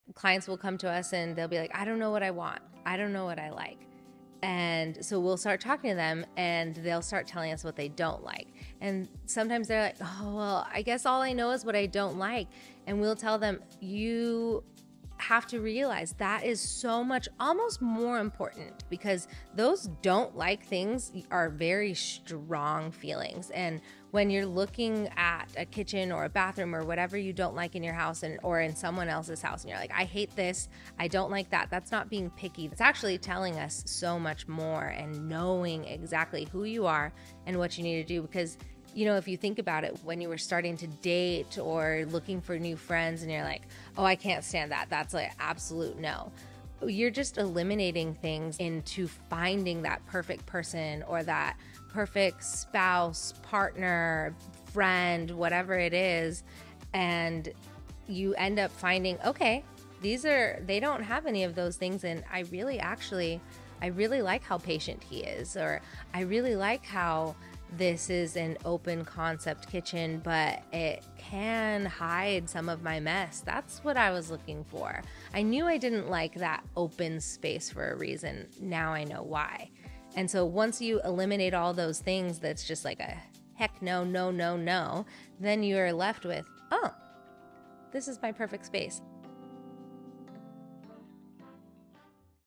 KM Design Details - Turning Your "No's" in the Best Yes
Understanding your "No's" is our secret weapon in designing your dream home. When clients come to us unsure of what they want, we start by identifying what they don't want. These strong feelings help us tailor a space that's perfect for you. It's not about being picky; it's about precision. We use your dislikes to create a home that reflects your unique style and needs, ensuring every detail is just right. Trust our expertise to turn your "No's" into the home of your dreams.